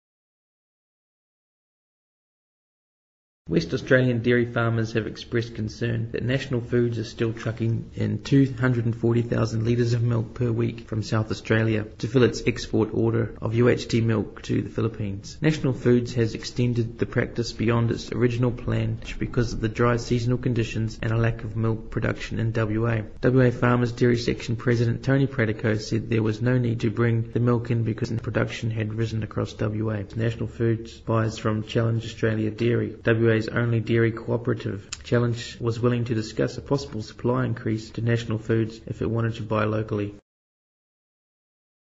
Lionco Lion Dairy and Drink Milks Western Australian RU@RISK Farmers
Milk for $1 per litre how long will it last? Lion Co
More National foods fruit and milk workers are set to lose their jobs as the  growing cancer called National foods further decimate their so called valued employees  Australia wide 3000 at last count and gaining.
All for increased profits to japanese owners and loss to Australia Food security.
Lion Dairy and Drink Australia  Dumps milk in Western Australia   to force farmers to take below cost of production
 prices  for their  hard work  and milk
Food Inc  methods seem to be the norm iin this case.
National foods destroyin Australian Lives election 2010Support  TRUTHINLABELLING.COM.AU   Boycott National Foods PURA MILK Brands and Berri Juice and drink Brands and help fellow Australianssee more at the youtube channel thewatcher8271